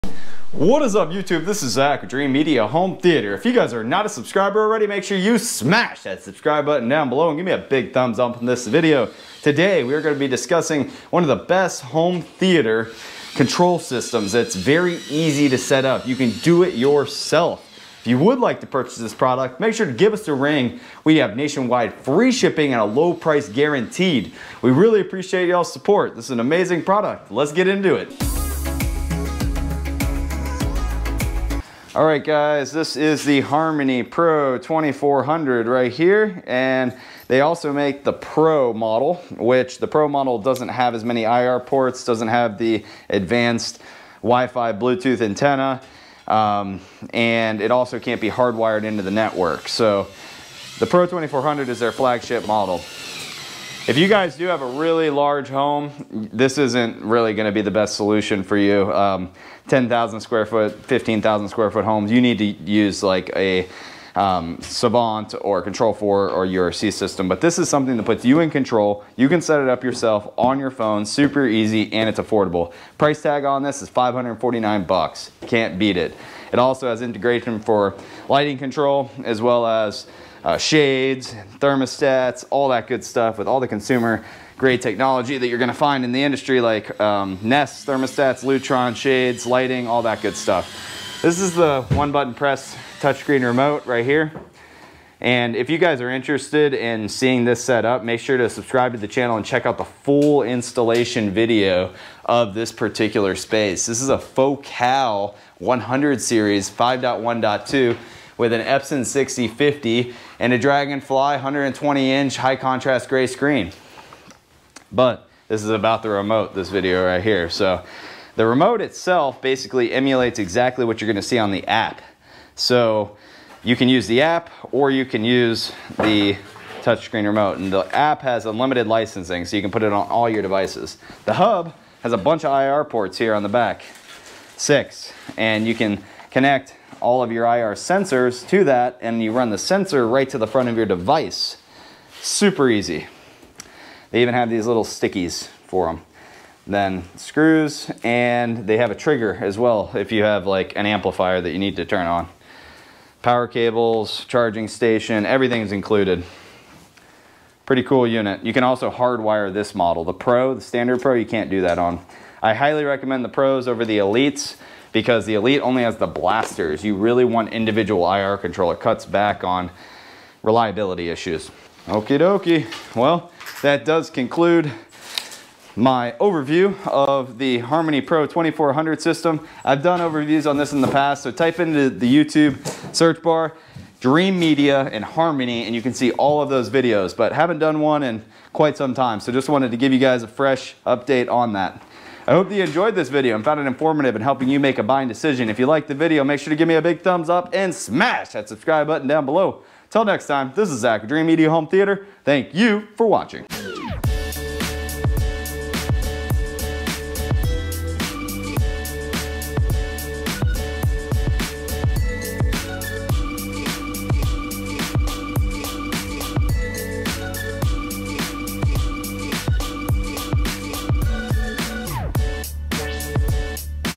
Best Universal Remote for Home Theater: Harmony Pro 2400
Harmony pro 2400
THE MOST ADVANCED HARMONY EXPERIENCE YET
Logitech Harmony Pro 2400 provides your client with the most advanced Harmony experience yet. An all-new hub with extended range antenna, Ethernet with PoE, and six 3.5mm IR ports; plus a scrollable touch screen remote and home control buttons to simplify the entertainment and home control experience.